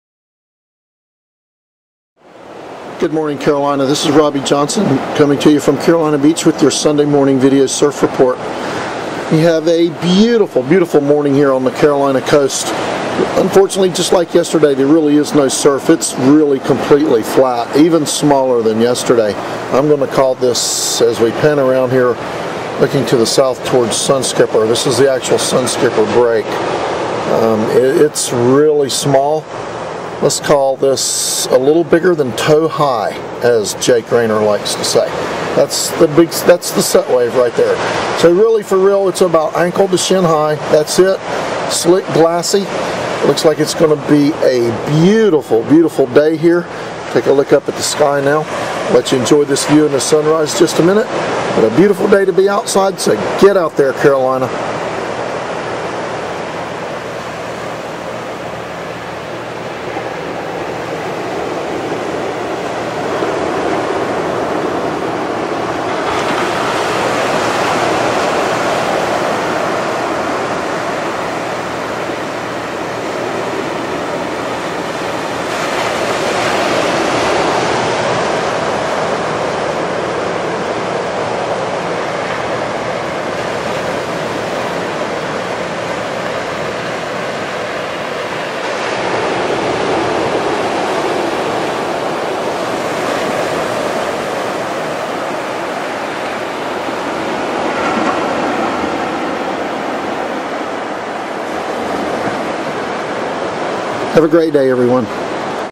Sunday report
AM surf report from Carolina Beach NC